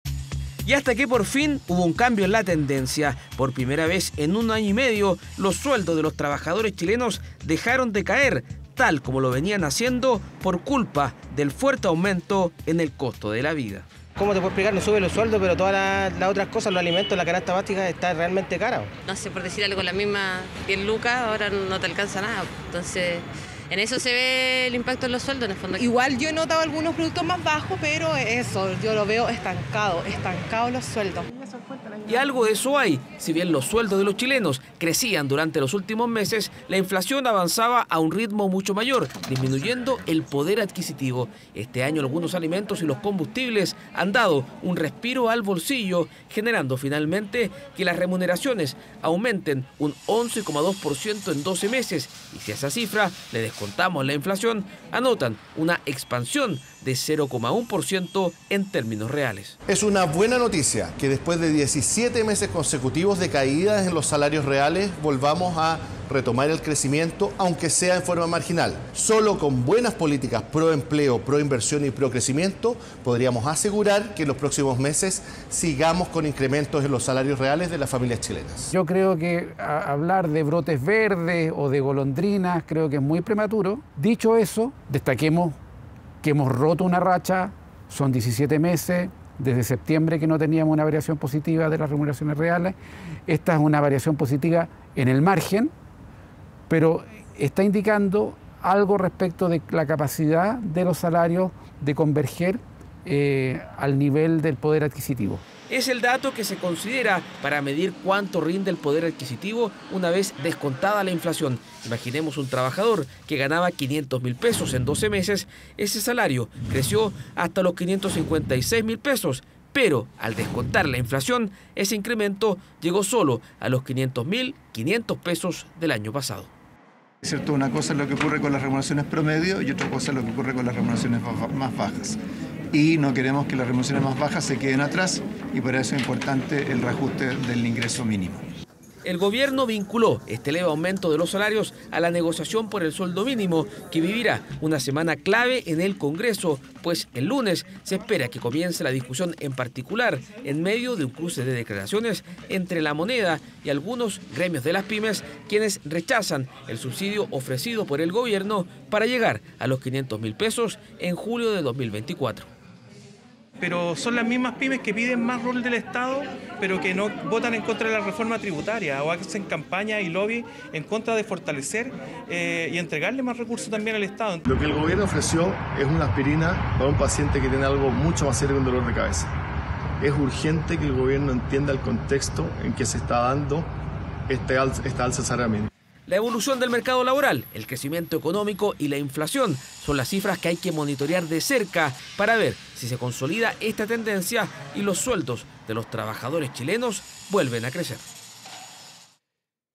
Después de 17 meses: por fin volvieron a subir los sueldos
Y esta es una buena noticia: los sueldos de los trabajadores chilenos, por fin volvieron a crecer. Tras un año y medio de sólo caídas debido a la inflación, en marzo hubo un leve aumento que quebró la tendencia.